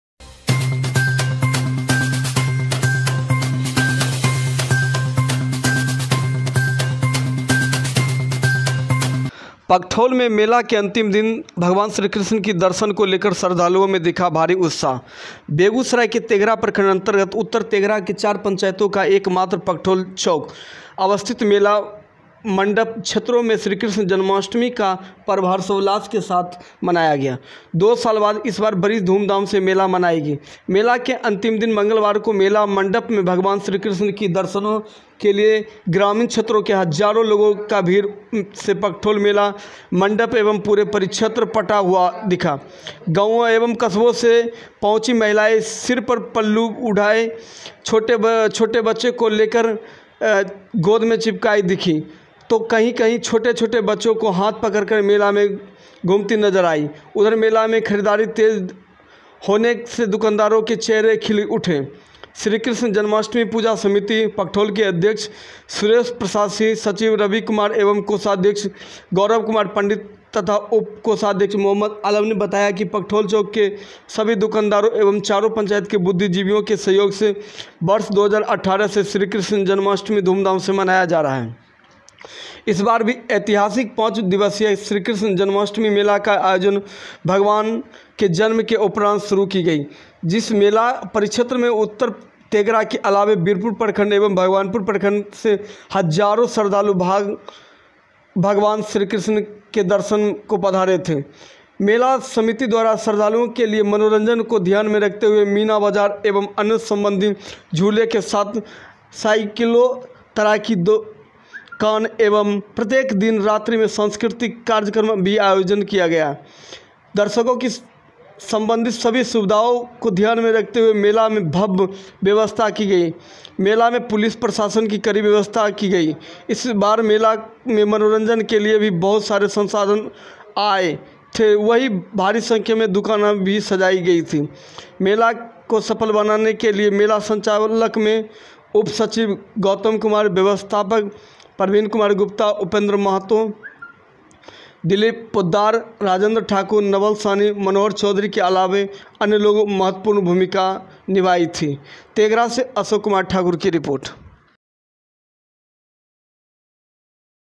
पकठौल में मेला के अंतिम दिन भगवान श्री कृष्ण की दर्शन को लेकर श्रद्धालुओं
पकठौल में मेला के अंतिम दिन भगवान श्री कृष्ण की दर्शन को लेकर श्रद्धालुओं मे दिखा भारी उत्साह।
अशोक कुमार ठाकुर
, तेघड़ा ( बेगूसराय)
 तेघरा प्रखंड अंतर्गत उत्तर तेघरा के 4 पंचायतों का एक मात्र पकठौल चौक अवस्थित  मेला मंडप  क्षेत्रों में श्री कृष्ण जन्माष्टमी का पर्व हर्षोल्लास के साथ मनाया गया।  2 साल बाद इस बार बड़ी धूमधाम से मेला मनाई गई । मेला के अंतिम दिन मंगलवार को मेला मंडप में  भगवान श्री कृष्ण की  दर्शनों के लिए लिए ग्रामीण क्षेत्रों के हजारों लोगों का भीड़ से पकठौल मेला मंडप एवं पूरे परीक्षेत्र पटा हुआ दिखा। गांवों एवं कस्बों से पहुंची महिलाएं सिर पर पल्लू उठाए छोटे-छोटे बच्चों को गोद में चिपकाए दिखी। तो कहीं छोटे-छोटे बच्चों को हाथ पकड़े मेला में घूमती नजर आई।उधर मेला में खरीदारी तेज होने से दुकानदारों के चेहरे खिल उठे ।श्री कृष्ण जन्माष्टमी पूजा समिति पकठौल के अध्यक्ष सुरेश प्रसाद सिंह ,सचिव रवि कुमार एवं कोषाध्यक्ष गौरव कुमार पंडित तथा उप कोषाध्यक्ष मोहम्मद आलम ने बताया कि। पकठौल चौक के सभी दुकानदारों एवं चारों पंचायत के बुद्धिजीवियों के सहयोग से वर्ष 2018 से श्री कृष्ण जन्माष्टमी धूमधाम से मनाया जा रहा है। इस बार भी ऐतिहासिक पांच दिवसीय कृष्ण जन्माष्टमी मेला का आयोजन भगवान के जन्म के उपरांत शुरुआत की गई। जिस मेला परिक्षेत्र में उत्तर तेघरा के अलावे बीरपुर प्रखंड एवं भगवानपुर प्रखंड से हजारों श्रद्धालु भगवान के दर्शन को पधारते हैं। मेला समिति द्वारा श्रद्धालुओं के लिए मनोरंजन को ध्यान में रखते हुए मीना बाजार एवं अन्य संबंधी झूले के साथ साइक्लो तरह की दुकान एवं प्रत्येक दिन रात्रि में सांस्कृतिक आयोजन किया जा रहा है साथ ही दर्शकों की संबंधित सभी सुविधाओं को ध्यान में रखते हुए मेला मैं भव्य व्यवस्था की गई है।मेला में पुलिस प्रशासन की कड़ी व्यवस्था की गई है इस बार मेला में मनोरंजन के लिए भी बहुत सारे संसाधन आए हे वही भारी संख्या में दुकानें भी सजाई गई है। मेला के सफल संचालन में उप सचिव गौतम कुमार व्यवस्थापक प्रवीण कुमार गुप्ता , उपेंद्र मेहता,दिलीप पोद्दार ,राजेंद्र ठाकुर ,नवल सहनी मनोहर चौधरी के अलावे अन्य लोगों की महत्वपूर्ण भूमिका रही।